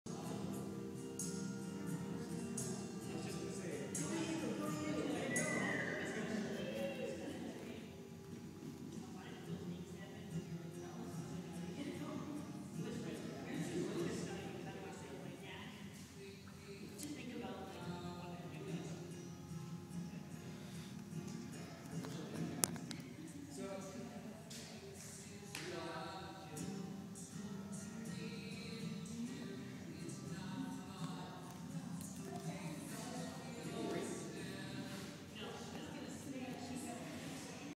First Pole Lab
Swinging on the pole during our first pole lab class, getting down basic moves. (I'm on the front pole in the black top.)